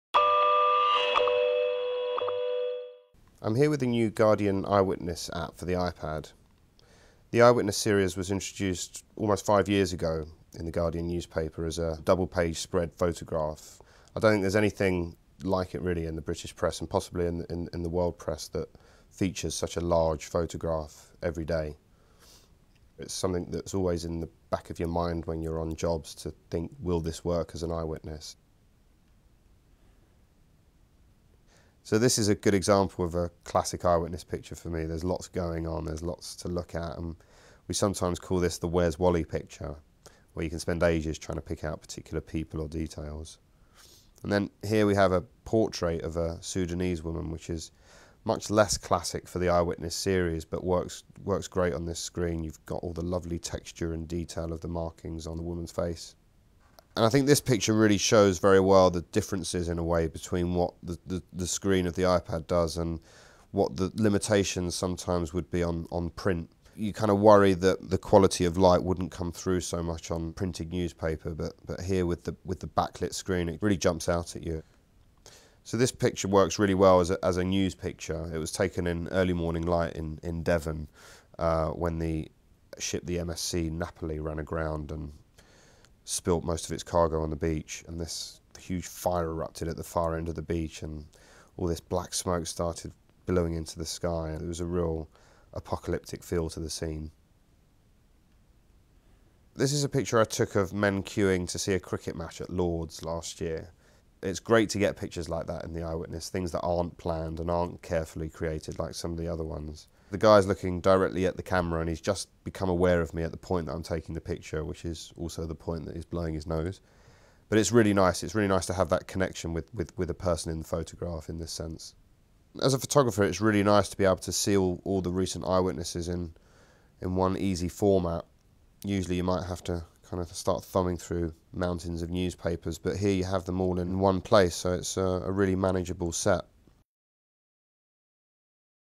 The Guardian's iPad Eyewitness app demonstrated by photographer David Levene
David Levene shows off the Guardian Eyewitness app for the Apple iPad